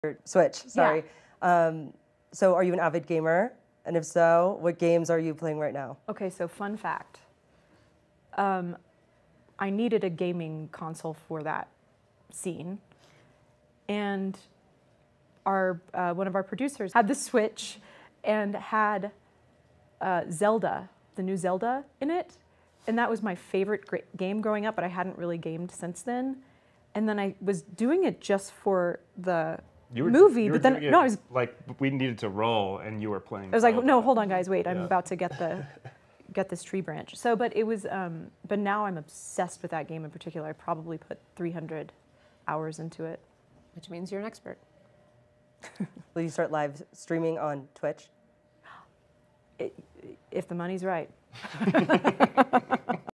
Zelda: Breath of the Wild - Indie Rock Artist St. Vincent 300 Hrs Playing BotW!! (Variety Interview)
Hi guys JohnoDaz(BringthePain) here, during an Interview with Variety the Indie Rock Artist St. Vincent mentioned how she put in over 300+ Hours Playing Zelda: BotW I thought this was really cool stuff see what you think much love guys!!